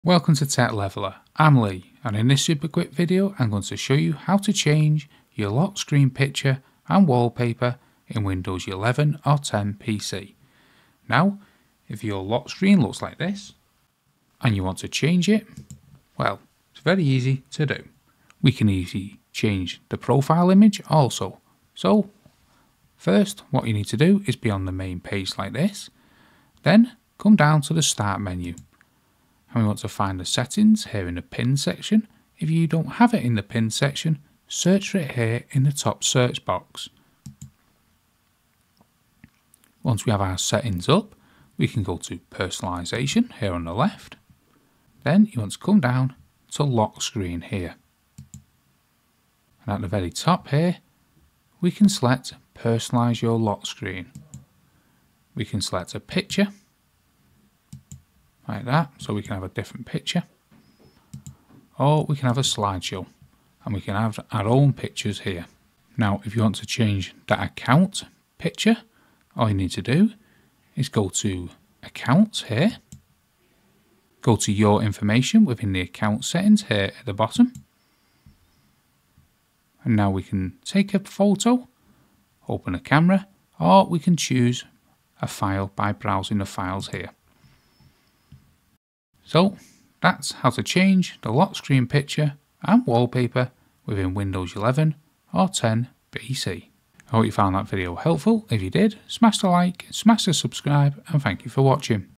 How to Change Lock Screen Picture & Wallpaper on Windows 11 or 10 PC
Learn How to Change Lock Screen Picture & Wallpaper on Windows 11 or 10 PC.

I hope this Windows video helped solve your problem. Is the Windows still having the problem?

0:00 Windows Help Intro
0:10 How to Change Lock Screen Picture & Wallpaper on Windows 11 or 10 PC